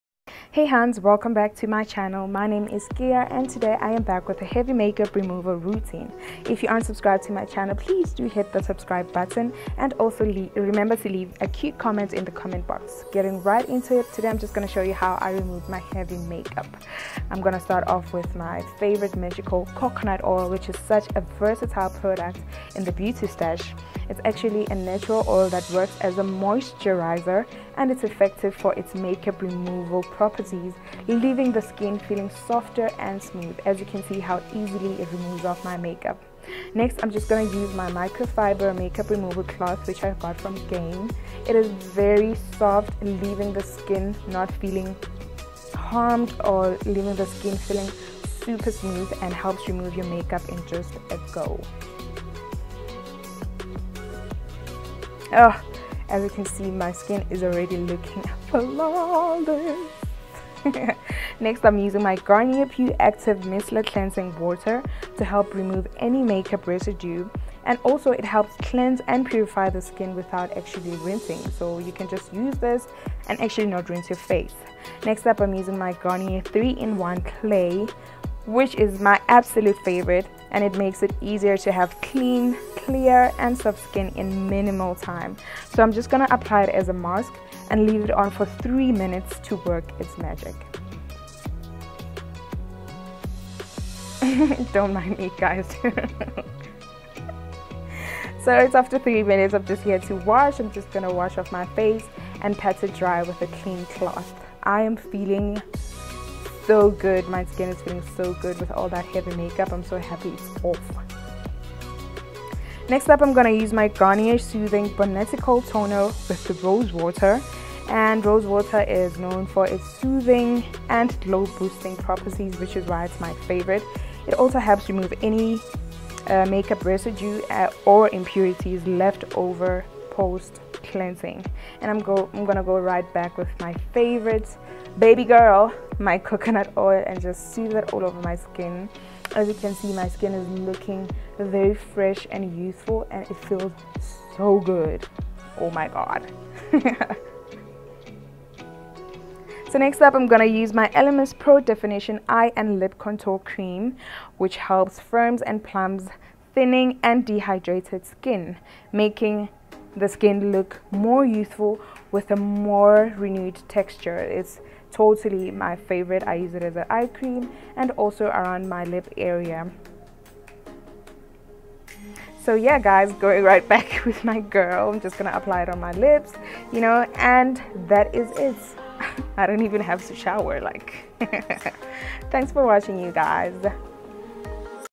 How I easily and safely remove heavy makeup in just 5mins!
Today's video is how I easily remove heavy makeup after a long day. I know it can feel like a chore but think of it as a skincare regime to keep your skin looking healthier and radiant. While most of us aren't as extreme with our makeup and skin care routines, it's still important to remove makeup safely without damaging the skin.

Missed my last videos? Watch it now

Songs
Musician: LiQWYD
Musician: LiQWYD